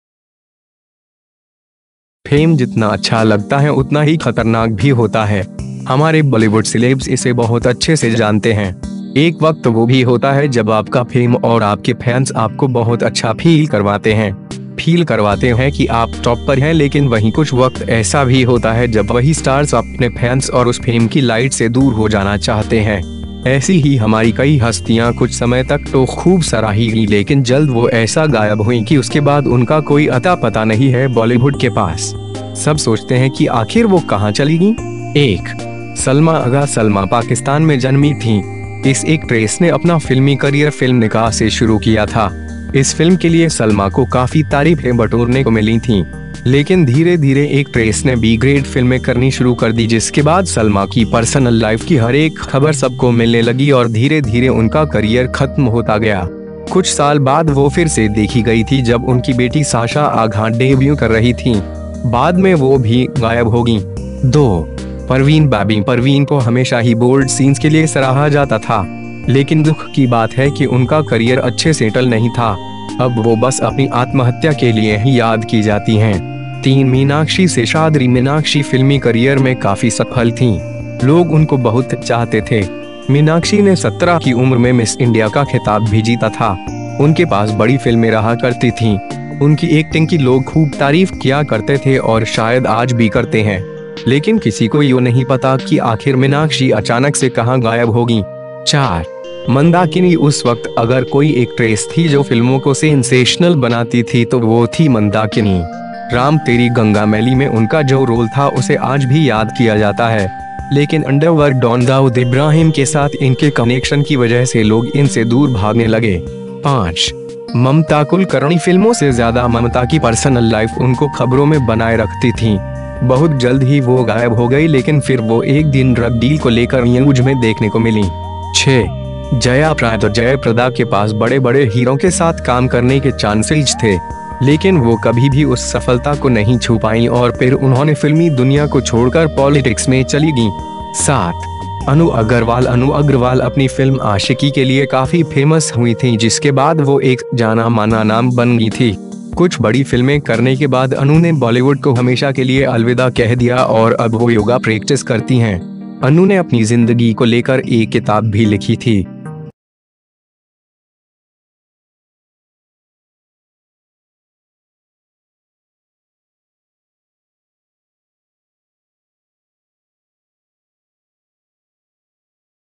कभी थे लोग दीवाने, अचानक परदे से गायब हुईं ये 7 एक्ट्रेसेस
फेम जितना अच्छा लगता है उतना ही खतरनाक भी होता है। हमारे बॉलीवुड सिलेब्स इसे बहुत अच्छे से जानते हैं। एक वक्त वो भी होता है जब आपका फेम और आपके फैंस आपको बहुत अच्छा फील करवाते हैं। फील करवातें है कि आप टॉप पर हैं लेकिन वहीं कुछ वक्त ऐसा भी होता है जब वही स्टार्स अपने फैंस और उस फेम की लाइट से दूर हो जाना चाहते हैं।

ऐसी ही हमारी कई हस्तियां कुछ समय तक तो खूब सराही गईं लेकिन जल्द वो ऐसा गायब हुईं कि उसके बाद उनका कोई अता पता नही है बॉलीबुड के पास। सब सोचते हैं कि आखिर वो कहां चली गईं?

1. Salma Agha
सल्मा पाकिस्तान में जन्मी थीं। इस एक्ट्रेस ने अपना फिल्मी करियर फिल्म निकाह से शुरू किया था। इस फिल्म के लिए सल्मा को काफी तारीफें बटोरने को मिलीं थीं। लेकिन धीरे-धीरे एक्ट्रेस ने बी-ग्रेड फिल्में करनी शुरू कर दी जिसके बाद सल्मा की पर्सनल लाइफ की हर एक खबर सबको मिलने लगी और धीरे-धीरे उनका करियर खत्म होता गया। कुछ साल बाद वो फिर से देखी गई थी जब उनकी बेटी साशा आघा डेब्यू कर रही थीं। बाद में वो भी गायब हो गईं।

2. Parveen Babi
परवीन को हमेशा ही बोल्ड सीन्स के लिए सराहा जाता था। लेकिन दुख की बात है कि उनका करियर अच्छे सेटल नहीं था। अब वो बस अपनी आत्महत्या के लिए ही याद की जाती हैं।

3 Meenakshi Seshadri
मिनाक्षी फिल्मी करियर में काफी सफल थीं। लोग उनको बहुत चाहते थे। मिनाक्षी ने 17 की उम्र में मिस इंडिया का खिताब भी जीता था। उनके पास बड़ी फिल्में रहा करती थीं। उनकी एक्टिंग की लोग खूब तारीफ किया करते थे और शायद आज बी करते हैं। लेकिन किसी को यो नही पता कि आखिर मिनाक्षी अचानक से कहां गायब हो गईं।

4 . Mandakini
उस वक्त अगर कोई एक्ट्रेस थी जो फिल्मों को सेंसेशनल बनाती थी तो वो थी मंदाकिनी। राम तेरी गंगा मैली में उनका जो रोल था उसे आज भी याद किया जाता है। लेकिन अंडरवर्ड डॉन दाउद इब्राहिम के साथ इनके कंनेक्शन की वजह से लोग इनसे दूर भागने लगे।

5. Mamta Kulkarni
फिल्मों से ज्यादा ममता की पर्सनल लाइफ उनको खबरों में बनाए रखती थीं। बहुत जल्द ही वो गायब हो गई लेकिन फिर वो एक दिन ड्रग डील को लेकर न्यूज में देखने को मिलीं।

6. Jaya Prada
जय प्रदा के पास बड़े बड़े हीरों के साथ काम करने के चान्सेज थे। लेकिन वो कभी भी उस सफलता को नहीं छू पाईं और पिर उन्होंने फिल्मी दुनिया को छोड़कर पॉलिटिक्स में चली गईं।

7. Anu Agarwal
अनू अग्रवाल अपनी फिल्म आशिकी के लिए काफी फेमस हुईं थीं जिसके बाद वो एक जाना माना नाम बन गईं थी। कुछ बड़ी फिल्में करने के बाद अनू ने बॉलीवुड को हमेशा के लिए अलविदा कह दिया और अब वो योगा प्रेक्टिस करती हैं। अनू ने अपनी जिंदगी को लेकर एक किताब भी लिखी थी।